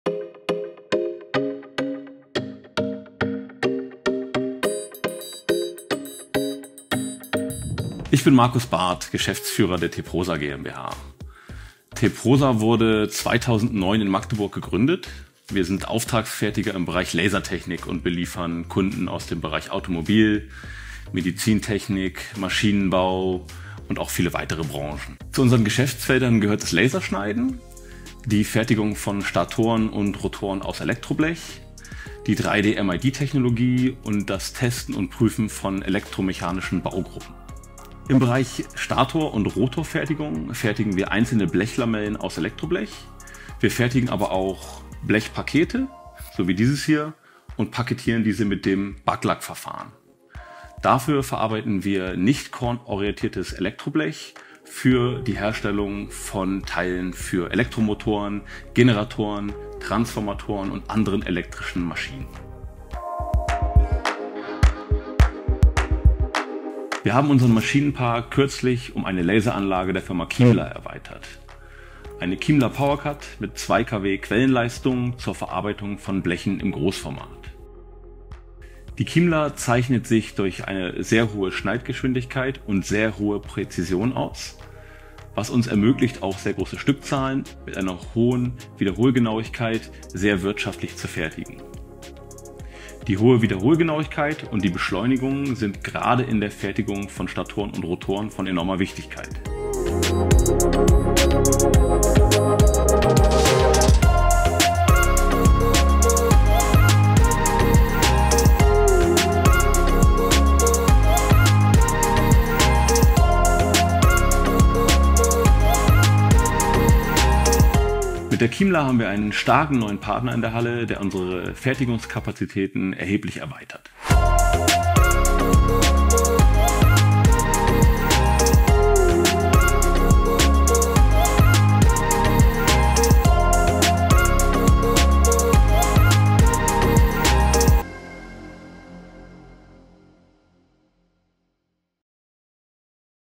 Laserschneiden von Elektroblech und Feinblech: Kundenstimme TEPROSA & KIMLA
TEPROSA fertigt in Magdeburg Lamellen aus Elektroblech für den Aufbau von Magnetkernen in Form von Rotoren, Statoren oder Transformatoren. Beim Laserschneiden verwendet das Unternehmen modernste Faserlaser und hat den Maschinenpark kürzlich um eine Laserschneidanlage von KIMLA erweitert. Im Video erzählt Geschäftsführer Markus Barth, warum TEPROSA sich für eine KIMLA-Laseranlage entschieden hat und welche Vorteile diese beim Laserschneiden von Feinblech und Elektroblech mit sich bringt.

ASKIAS ist exklusiver Vertriebspartner von KIMLA in Deutschland. Alle Informationen zu den Laserschneidanlagen von KIMLA gibt es

Zum YouTube-Kanal von TEPROSA: @teprosa3160